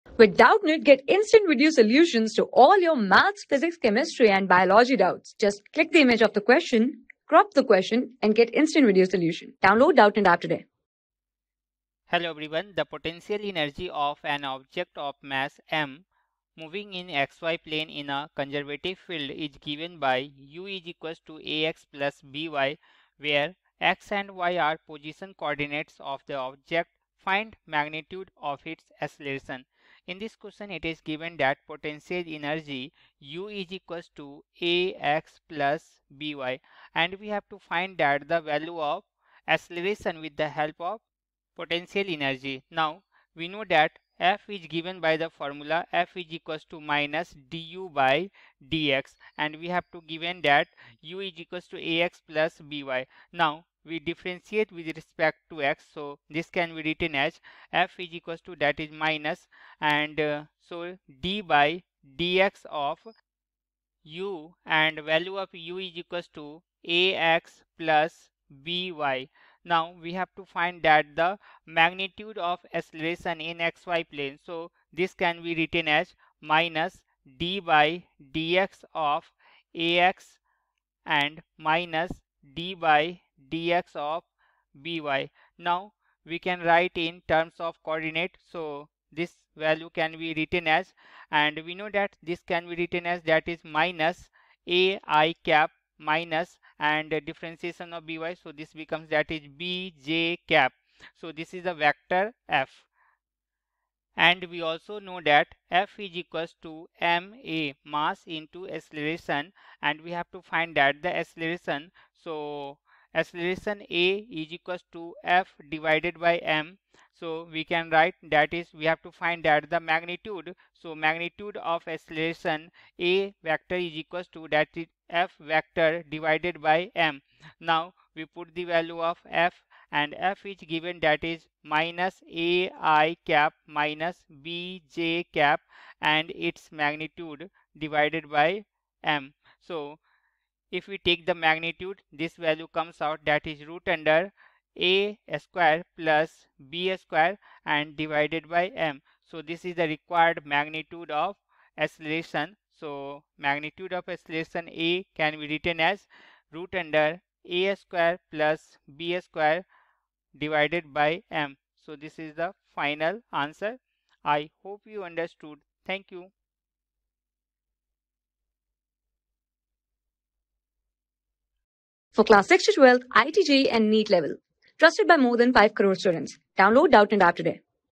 The potential energy of an object of mass m moving in xy plane in a conservative field is given ...
The potential energy of an object of mass m moving in xy plane in a conservative field is given by U = ax + by , where x and y are position coordinates of the object. Find magnitude of its acceleration :-
Class: 12
Subject: PHYSICS
Chapter: WORK, ENERGY AND POWER
Board:IIT JEE

👉 Have Any Query? Ask Us.